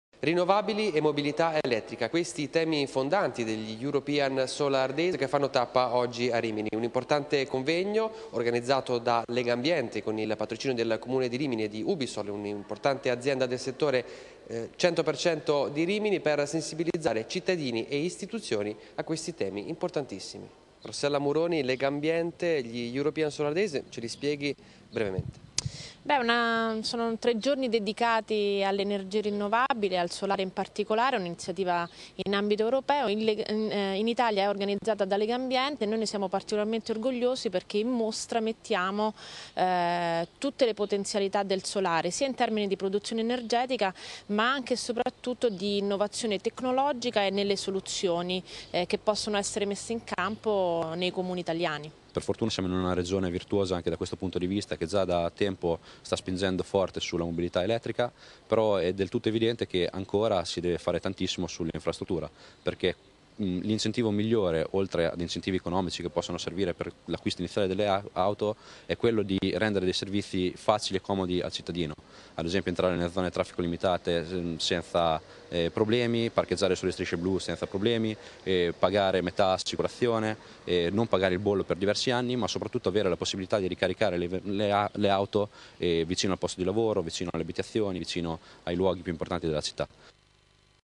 Partono da Rimini gli European Solar Days
Tutto pronto a Rimini per gli "European Solar Days", un'iniziativa che rientra nel programma europeo di sensibilizzazione all'utilizzo dell'energia solare.